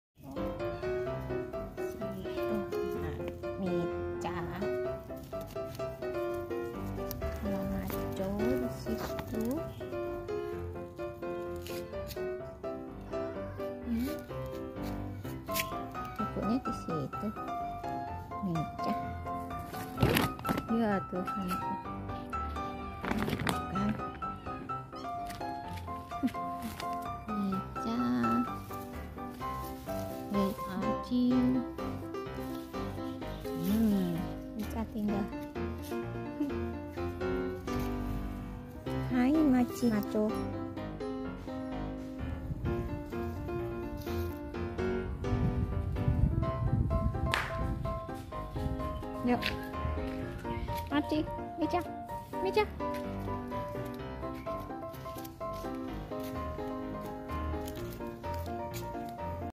Tiga anak kucing suka di pot tanaman,@kucingestichannel2864
Tiga anak kucing suka di pot tanaman. Anak kucing manis gembul, gembrot, gemoy, lucu, imut dan bikin gemas selalu menarik perhatian dan membuat kita ingin mengganggunya. Seperti tiga anak kucing kami yang suka atau senang bermain di pot tanaman. Rasanya ingin ikut santai bersama mereka menikmati kesegaran taman.